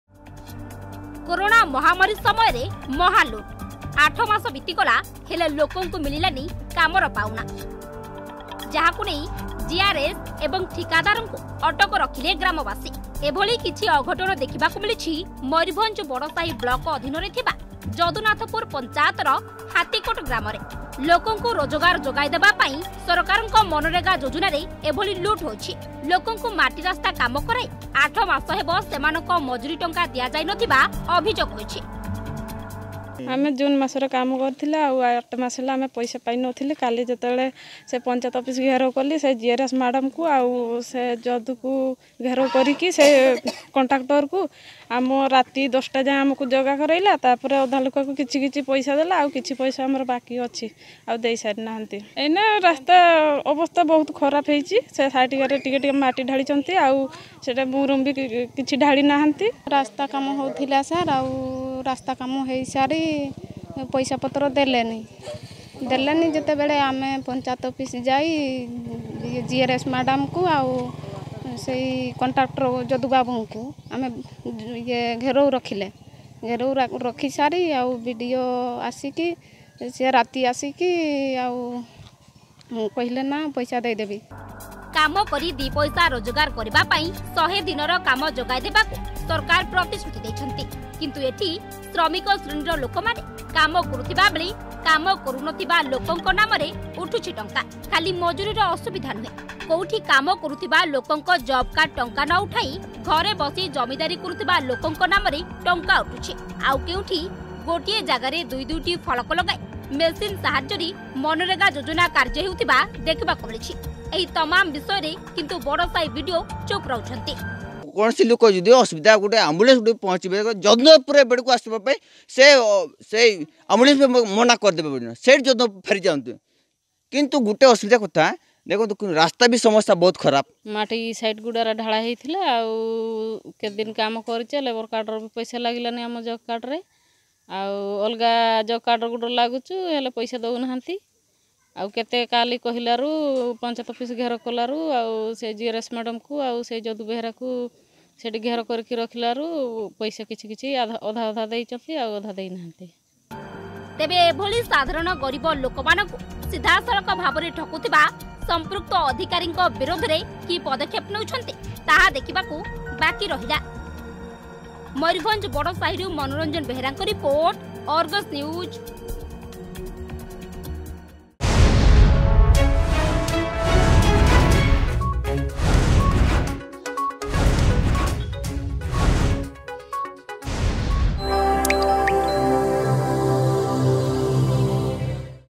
କୋରନା ସମୟରେ ମହାଲୁଟ୍
କୋରନା ସମୟରେ ମହାଲୁଟ୍‌ । 8 ମାସ ବିତିଗଲା ହେଲେ ଲୋକଙ୍କୁ ମିଳିଲାନି କାମର ପାଉଣା । ଯାହାକୁ ନେଇ ଜିଆର୍ଏସ୍ ଏବଂ ଠିକାଦାରଙ୍କୁ ଅଟକ ରଖିଲେ ଗ୍ରାମବାସୀ । ଏଭଳି କିଛି ଅଘଟଣ ଦେଖିବାକୁ ମିଳିଛି ମୟୂରଭଞ୍ଜ ବଡସାହି ବ୍ଲକ ଅଧିନରେ ଥିବା ଯଦୁନାଥପୁର ପଞ୍ଚାୟତର ହାତୀକୋଟ୍ ଗ୍ରାମରେ.